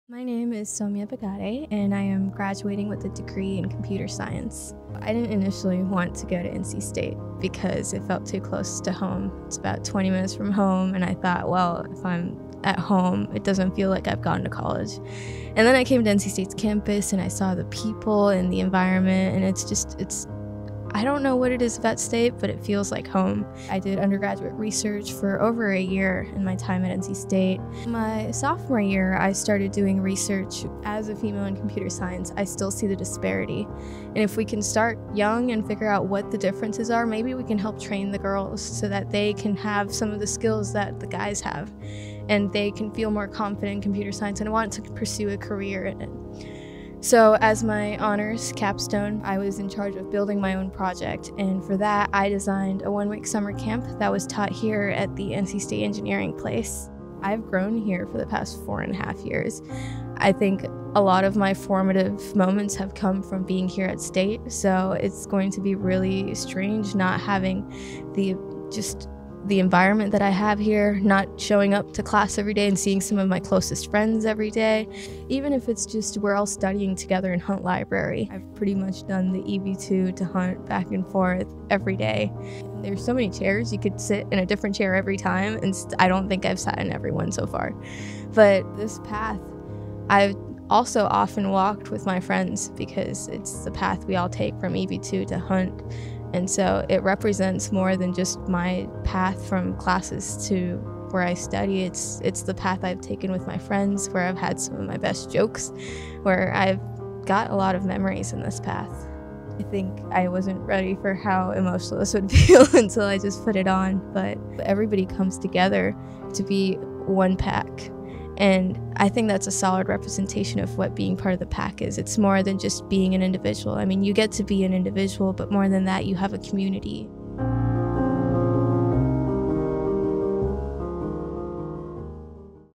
Cultivating a Campus Community: Soumya Bagade '19 | Pathway to Graduation
December grad Soumya Bagade talks about the path to earning her computer science degree from the NC State College of Engineering.